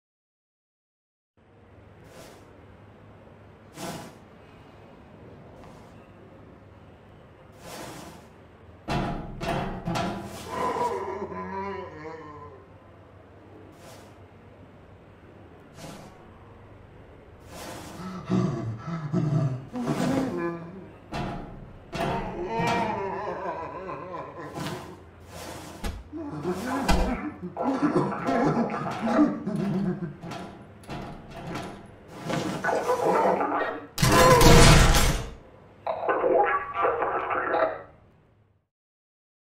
Necrotic | HL:A | MegaConstrux StopMotion Short
A Half Life StopMotion animation

Animation Software: Stopmotion studio pro

12 FPS